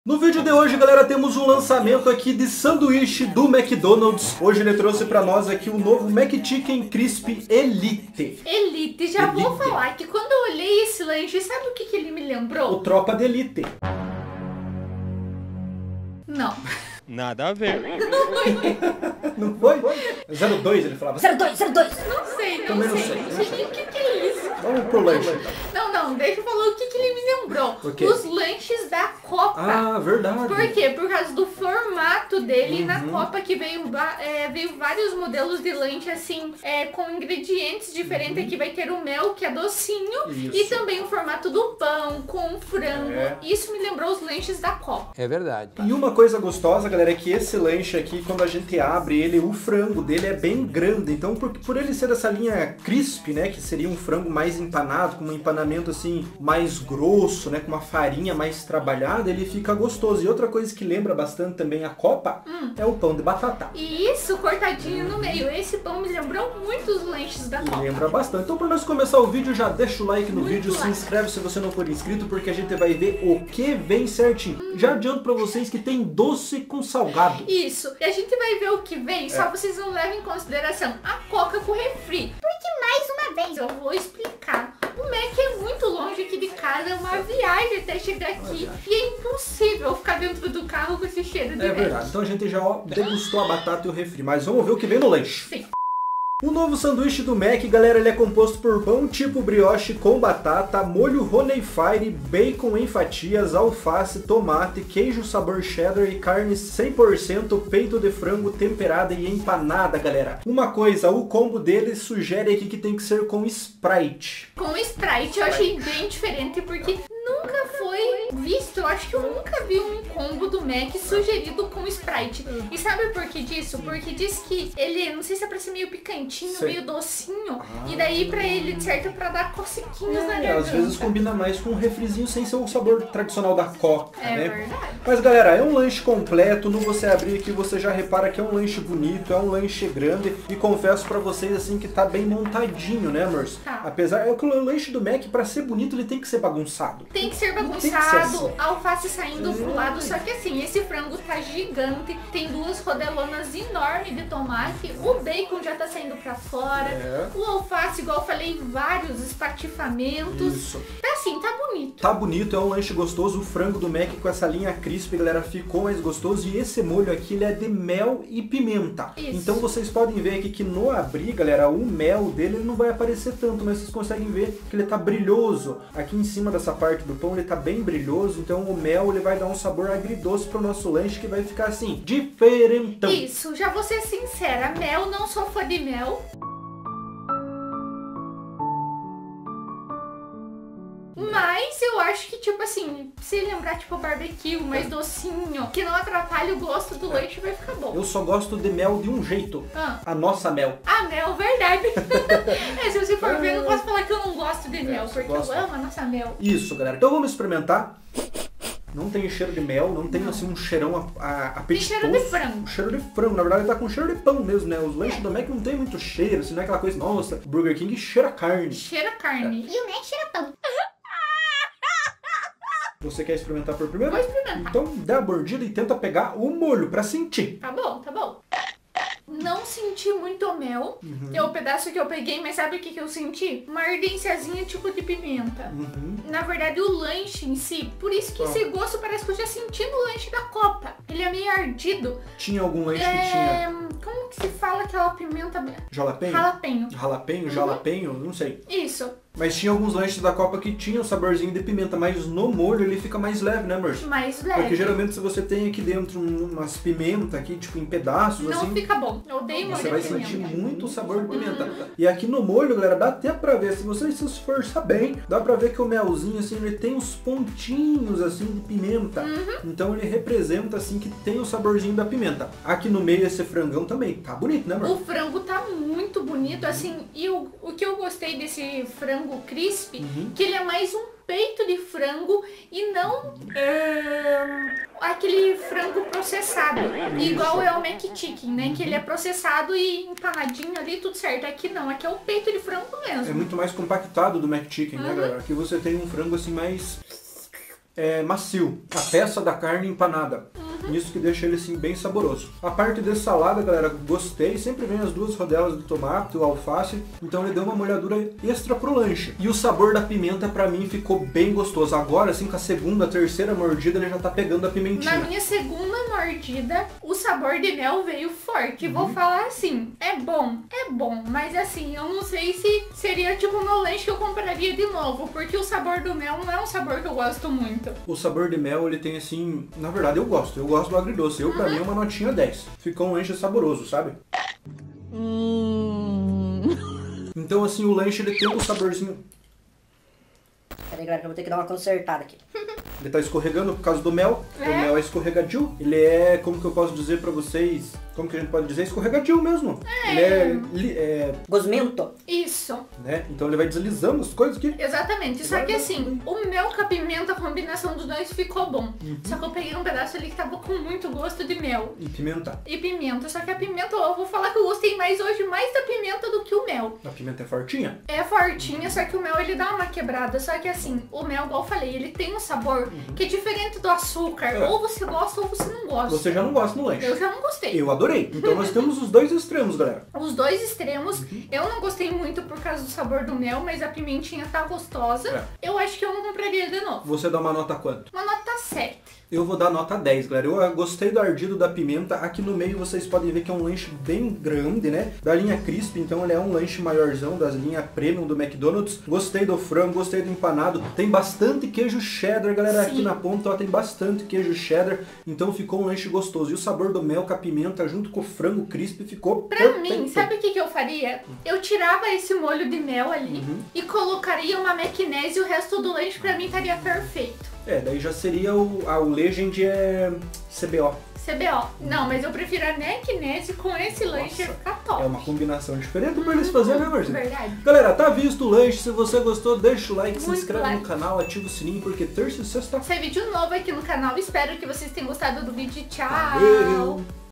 MCDONALD'S - NOVO McChicken Crispy ELITE com MEL E PIMETA - vale a pena?
O McDonald's acaba de lançar mais um lanche, e desta vez nós PROVAMOS O NOVO MCCHICKEN CRISPY ELITE, um lanche de frango crispy do MÉQUI que tem com diferencial o sabor do MEL E DA PIMENTA, será que esse novo lanche do mcdonalds vale a pena?

- Página no Facebook: Canal Joesile e Amanda